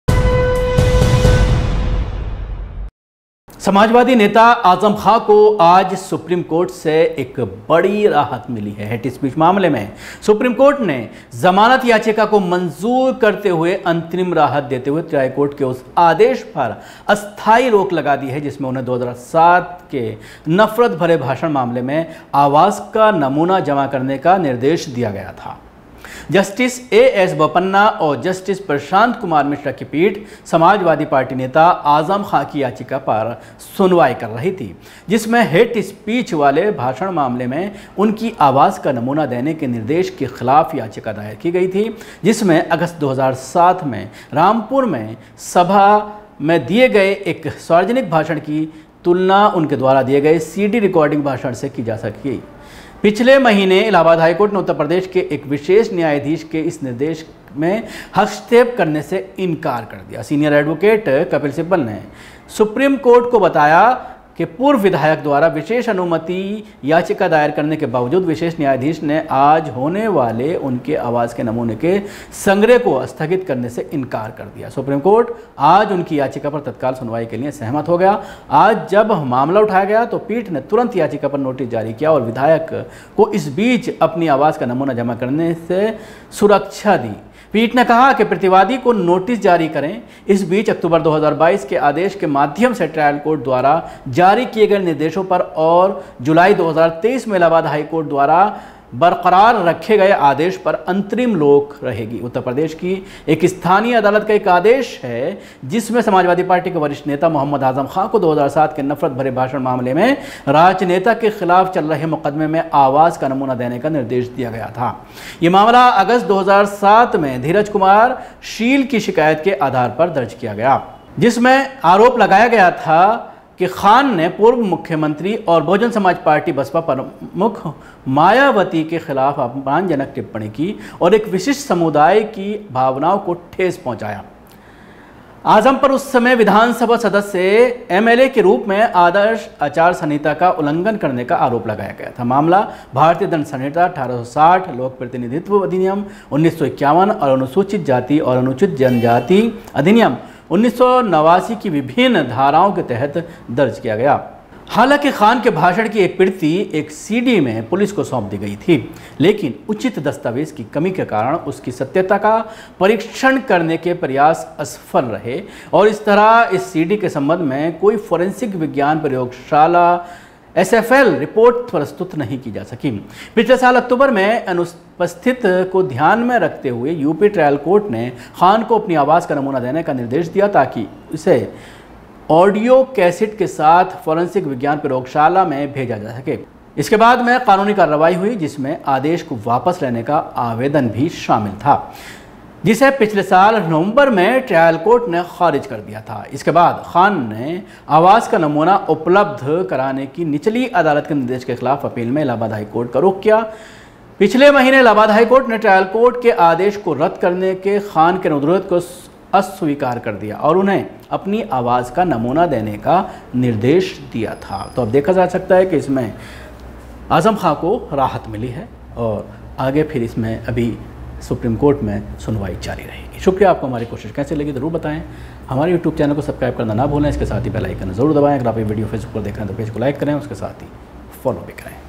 हेट स्पीच मामले में आज़म खान को SC से राहत इलाहाबाद हाई कोर्ट ने राहत देन से कर दिया था इनकार
हेट स्पीच मामले में आज़म खान को सुप्रीम कोर्ट से राहत इलाहाबाद हाई कोर्ट ने राहत देन से कर दिया था इनकार